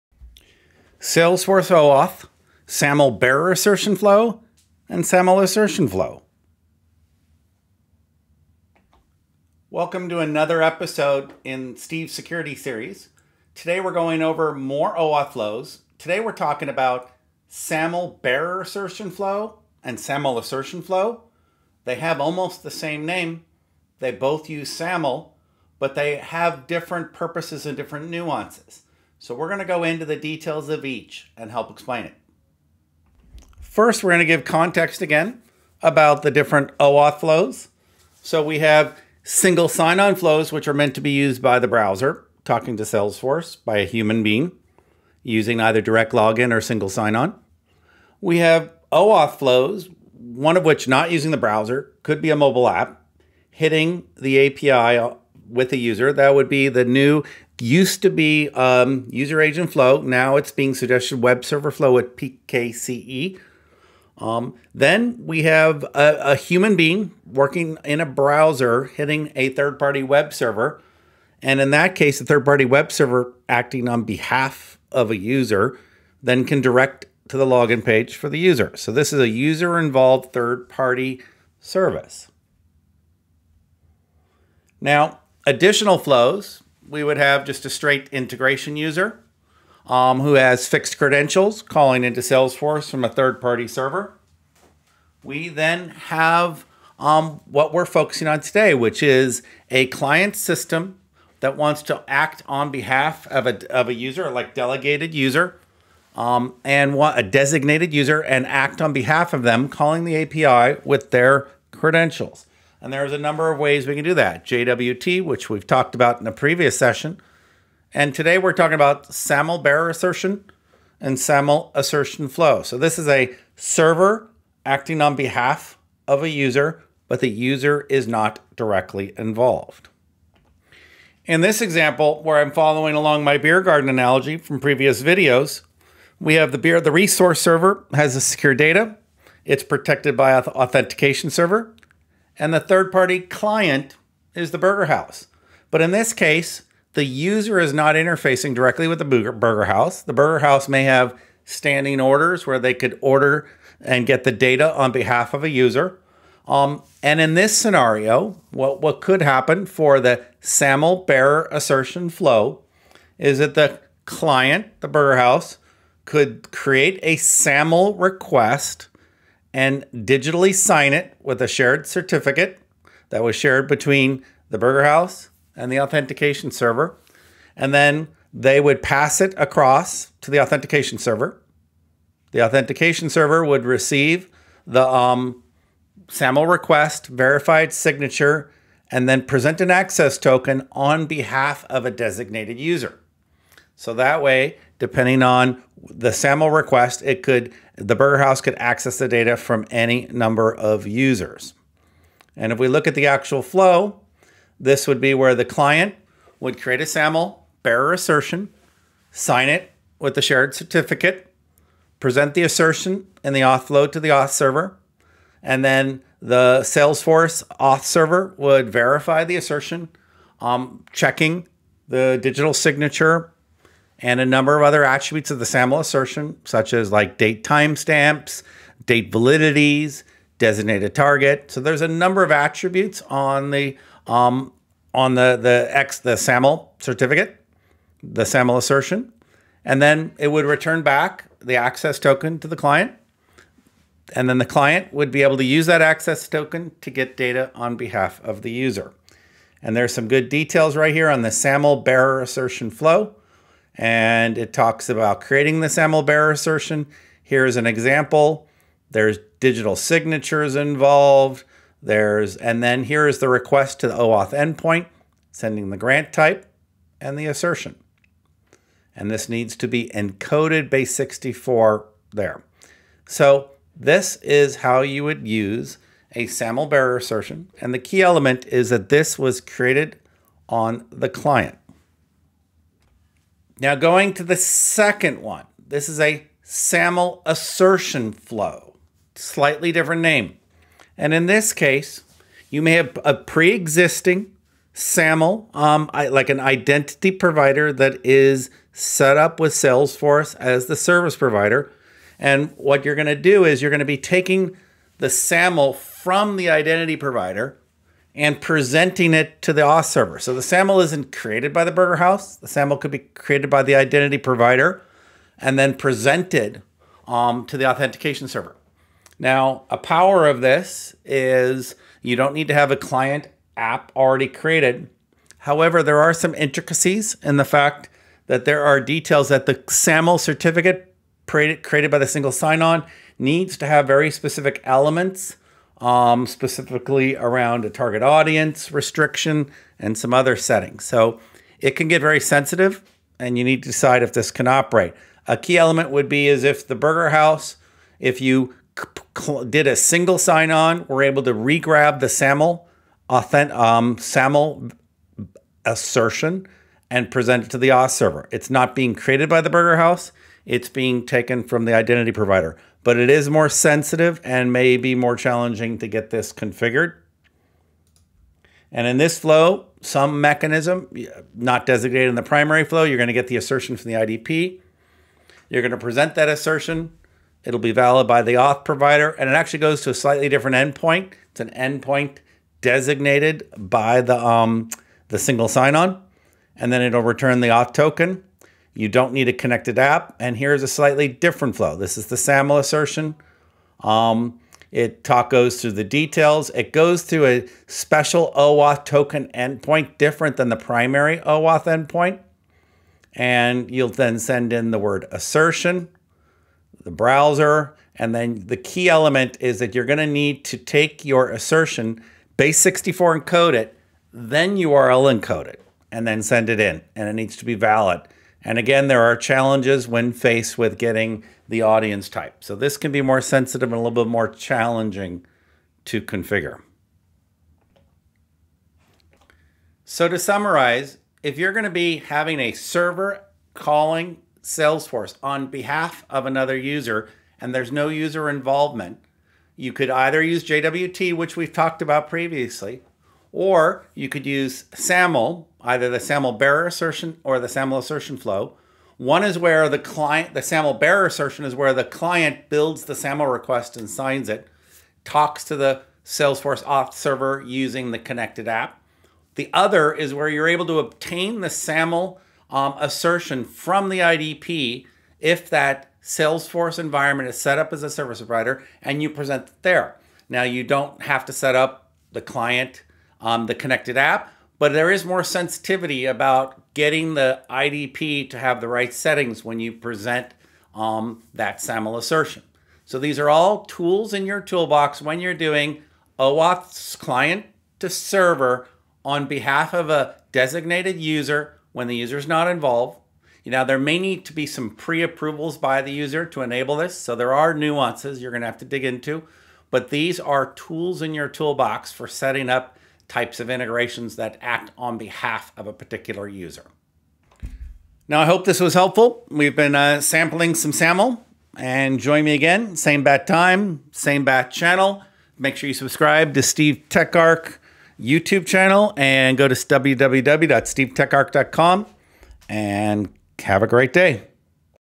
Salesforce OAuth SAML Bearer Assertion and SAML Assertion Flow
Dive into the world of OAuth flows with Steve's Security Spectacular Series. In this episode, we unravel the SAML Bearer Assertion Flow and the SAML Assertion Flow in Salesforce. While they might sound similar, they have unique nuances.

What You'll Learn:
The process of the SAML Bearer Assertion Flow.
Insights into the SAML Assertion Flow and its steps.
Key differences between the flows.

This video is in the Salesforce Security Spectacular.

STA 3.17